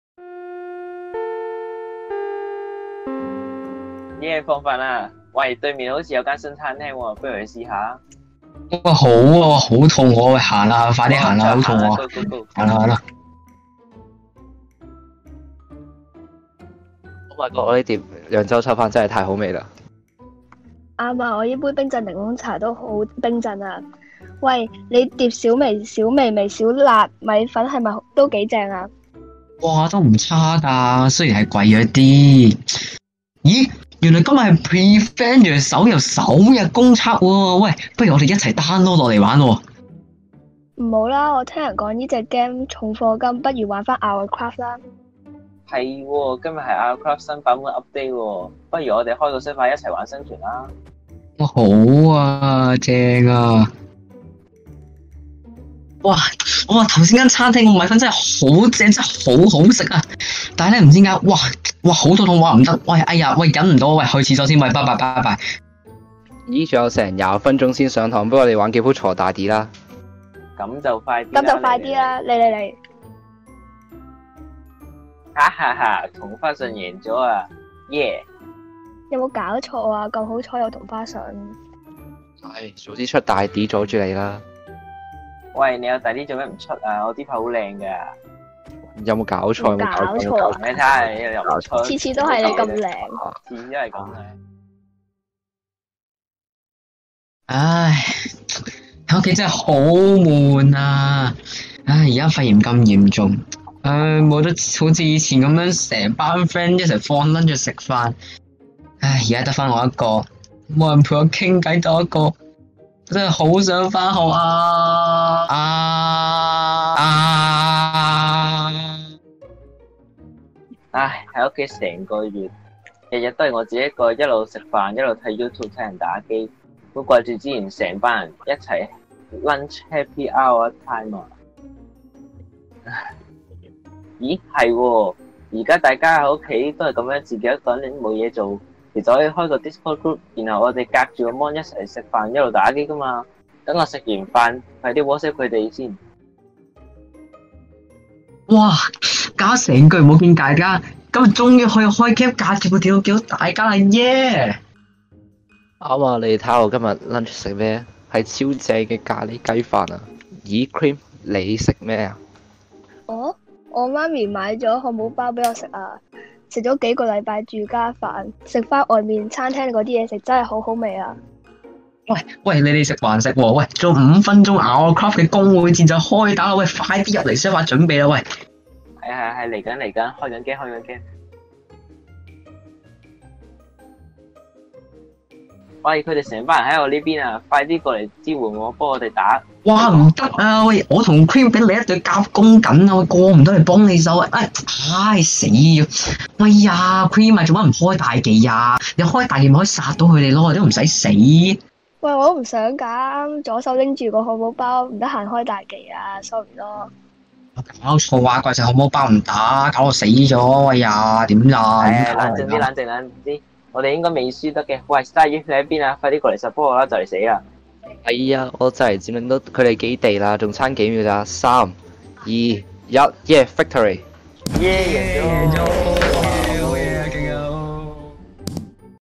48. Garson_Blue - 疫情Lunch Time
方案編號：48
方案名稱：疫情Lunch Time
隊伍名稱：Garson_Blue
投選你心目中的助人點子，入圍10隊將獲資助實踐方案!!
感創敢為2.0 – 青年社會創新服務獎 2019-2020　公眾投票
日期：7月13-19日